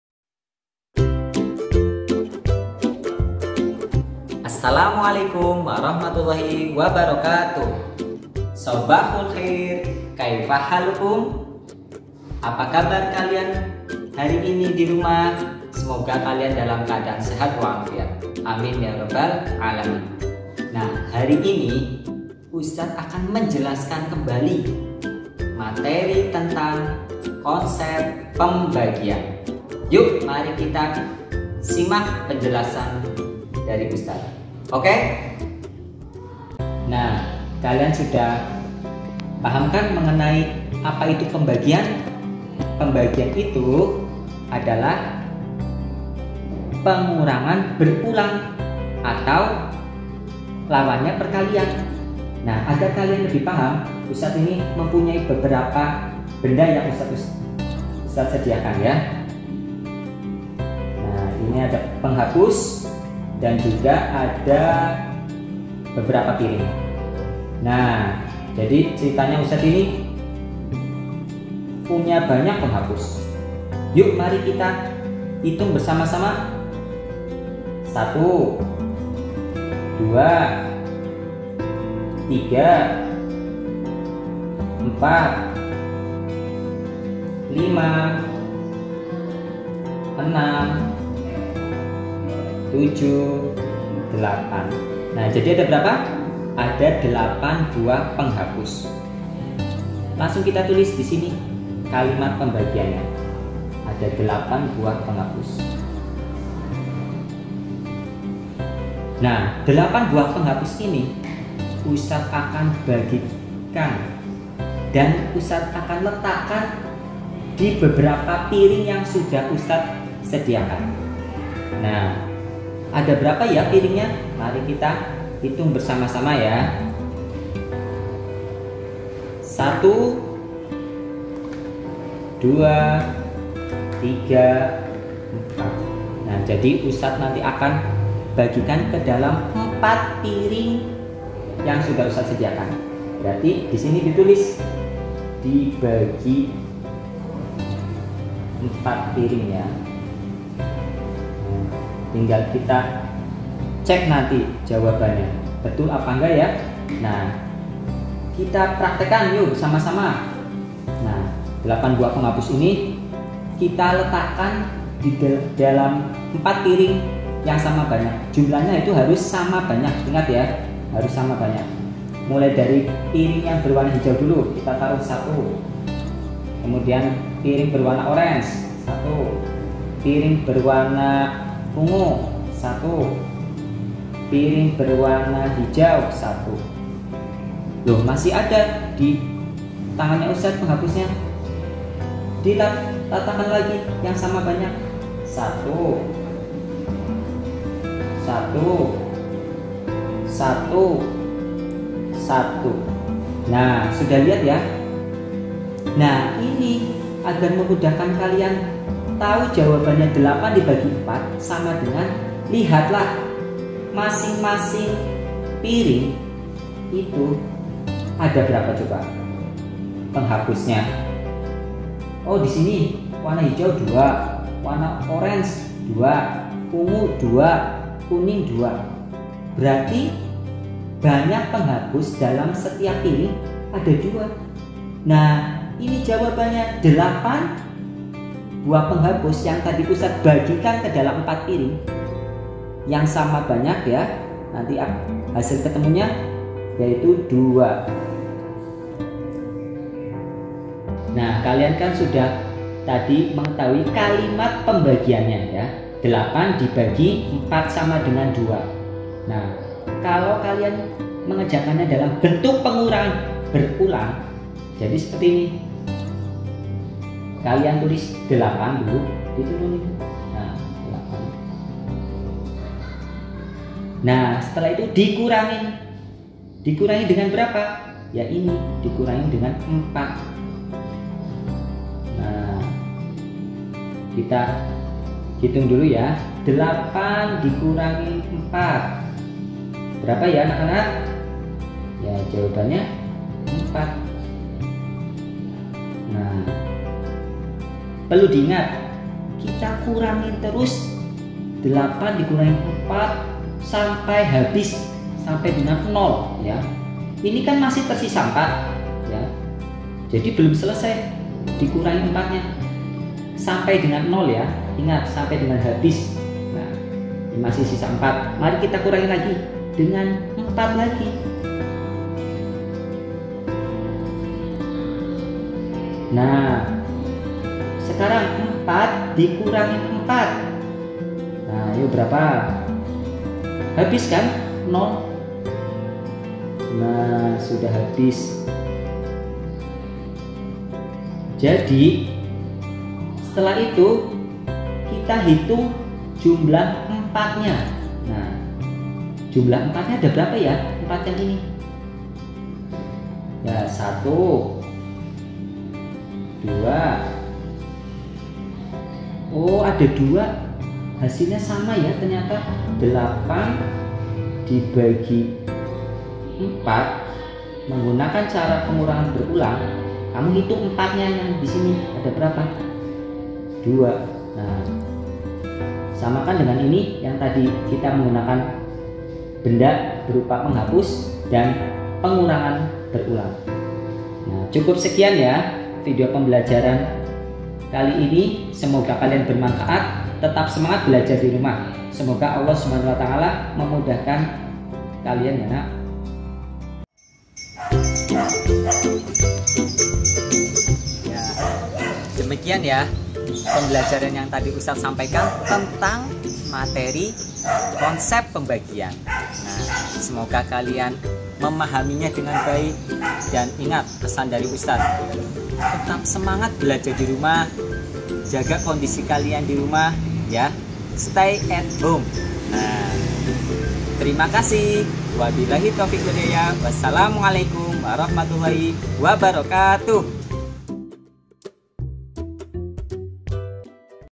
Konsep Pembagian | Kelas 2 | SD Al Irsyad 02 Purwokerto
Materi pembelajaran TEMATIK tentang konsep pembagian di kelas 2 SD Al Irsyad Al Islamiyyah 02 Purwokerto